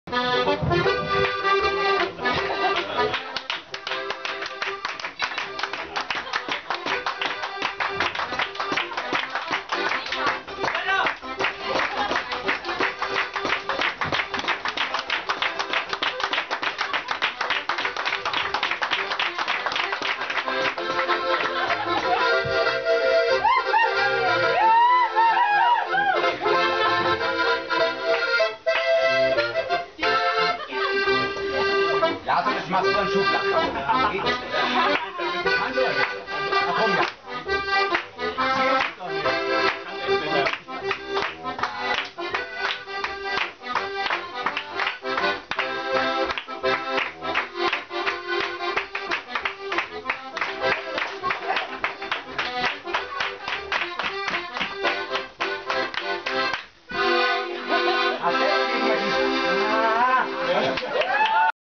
Ziehharmonikaspiel auf der Gschnagenhardtalm in Südtirol
Ziehharmonikaspiel auf der Gschnagenhardtalm im Naturpark Puez-Geisler, Vilnösstal. Parco Naturale Puez-Odle, Malga Casnago, Val di Funes.